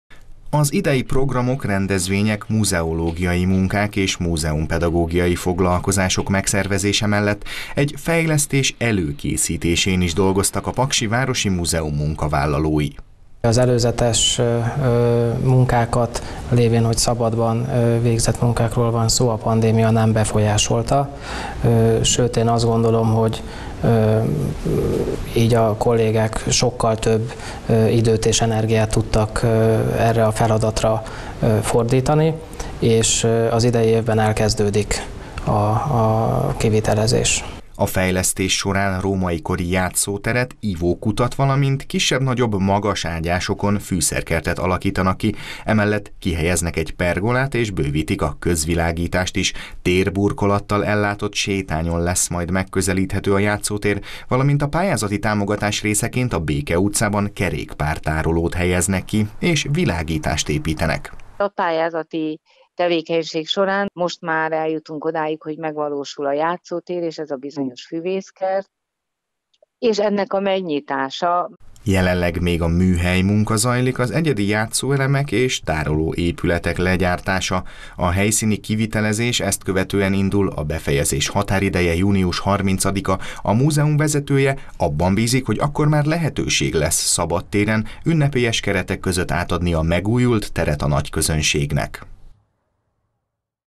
Hamarosan kezdődik a Lussonium fejlesztése
Jelenleg a műhelymunka zajlik, hamarosan elindul a Lussonium fejlesztése. Paks Város Önkormányzata konzorciumban nyert a Terület- és Településfejlesztési Operatív Program pályázatán turisztikai fejlesztésre. Mintegy 36 millió forintból római kori játszóteret és füvészkertet alakítanak ki.